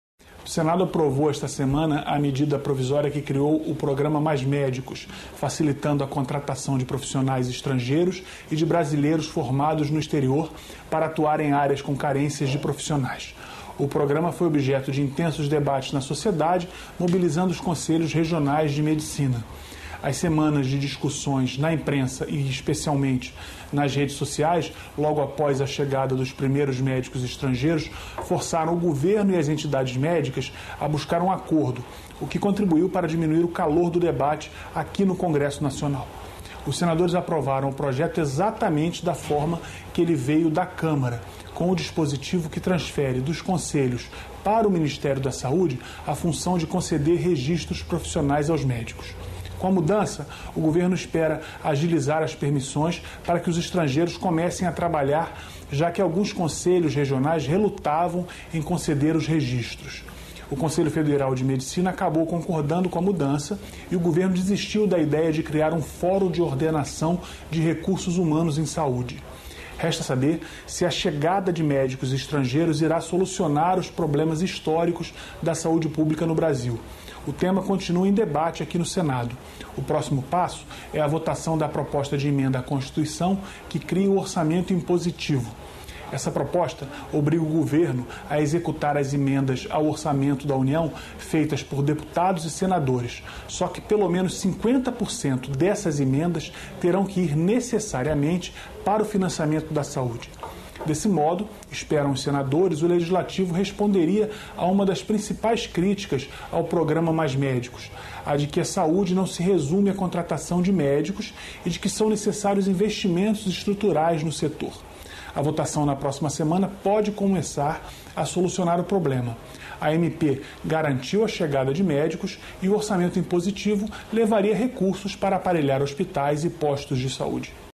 Aprovação da MP do Mais Médicos é o destaque da semana
A aprovação da MP que criou o programa Mais Médicos foi o principal destaque entre as votações da semana. Veja as informações com o coordenador da Agência Senado, Marco Antônio Reis.